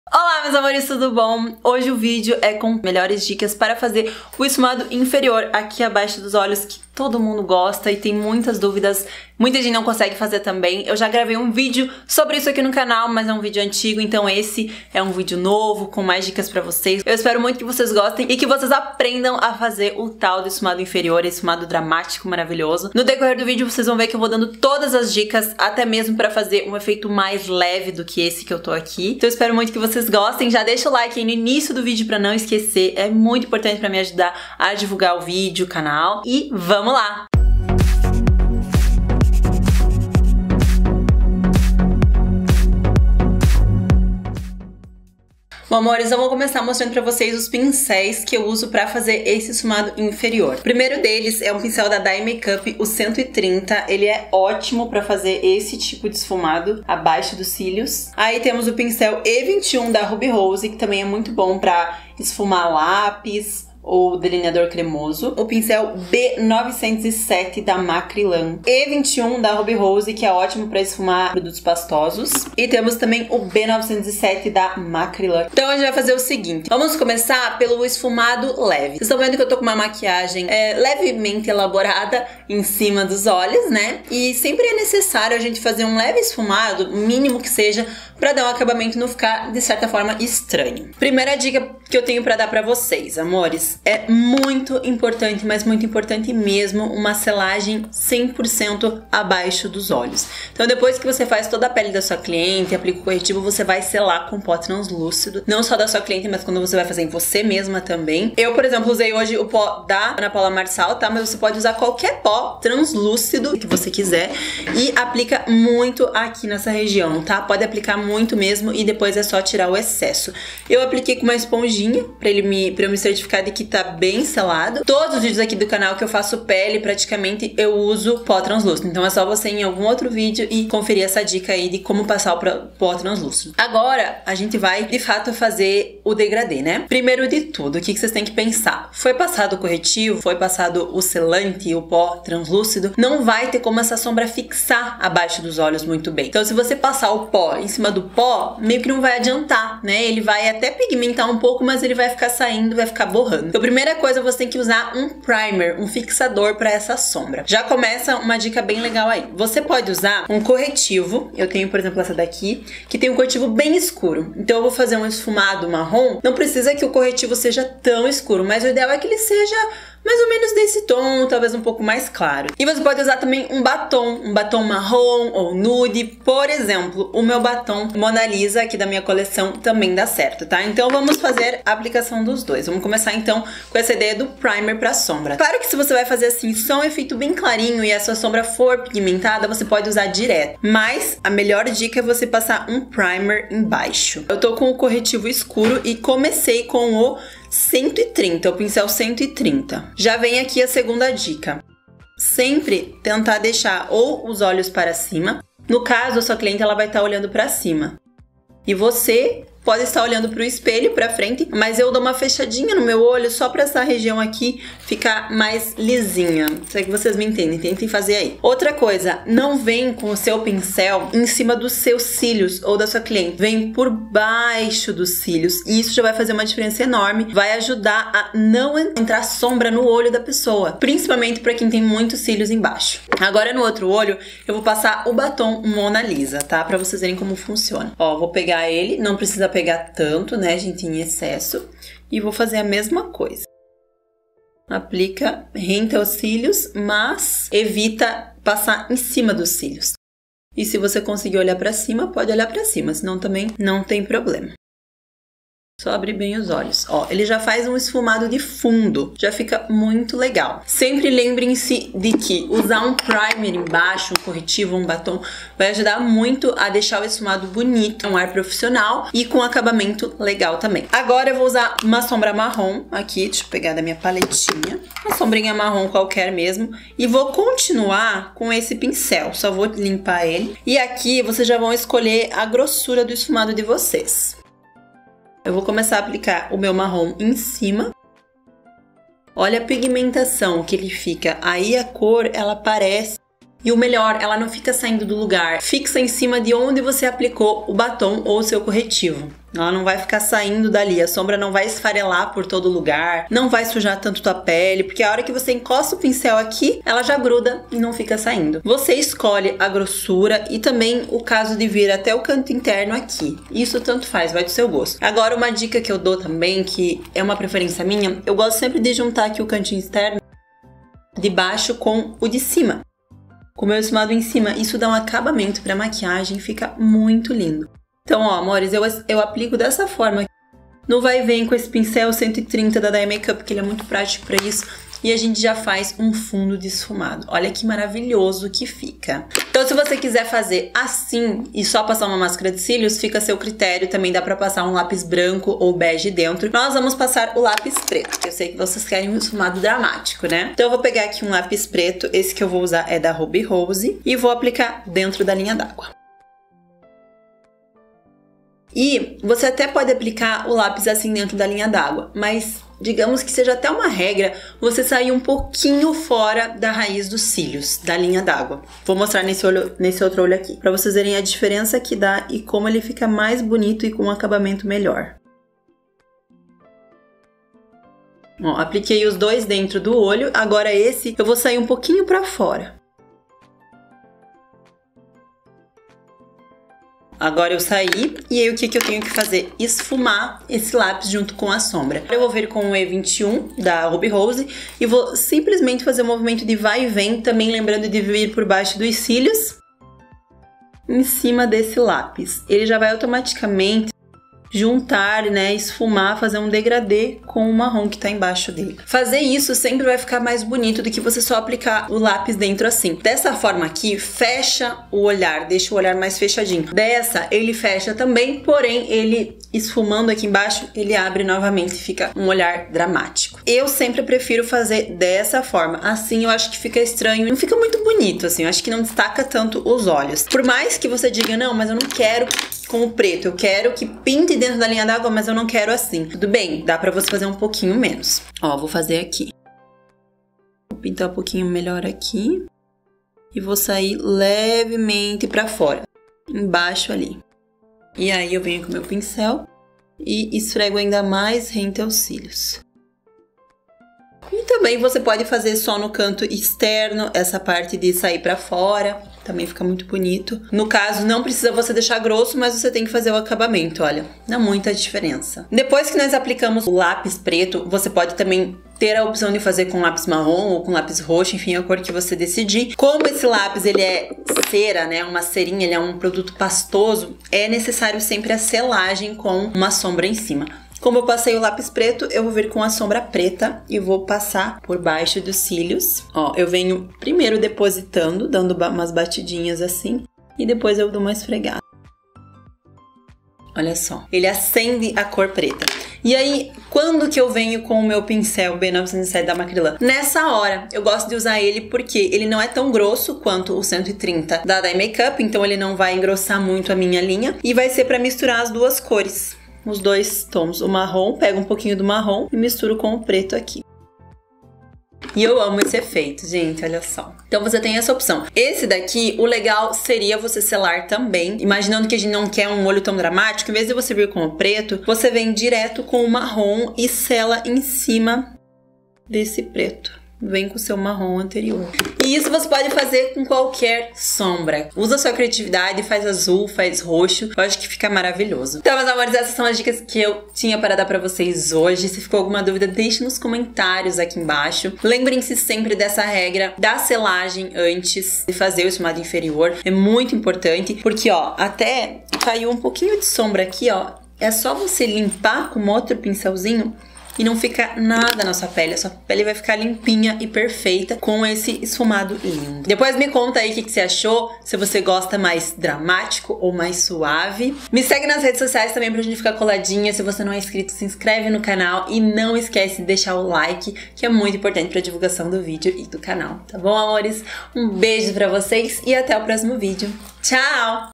MELHORES DICAS PARA FAZER ESFUMADO INFERIOR
Olá amores, o vídeo de hoje é uma versão atualizada de um outro vídeo mais antigo aqui do canal, nele eu mostro dicas importantíssimas para de fazer o temido esfumado inferior!!

 Siga também a loja no instagram: @cerejafloroficial

  Produtos usados:

01:49- Pó Translucido Nº 51 Ana Paula Marcal
02:44- Quarteto Corretivos Bitarra Beauty
02:57- Batom Líquido Matte Bárbara Thais - Lip Art Makeup cor-MonaLisa
05:52- Lápis Delineador Preto 91-Ruby Rose

Pinceis:
00:53- O130 Day MakeUp
00:59- E21 Ruby Rose
01:05- B-907 Macrilan:

 Siga também o perfil da minha marca de maquiagem: @lipartmakeup

Beijinhos!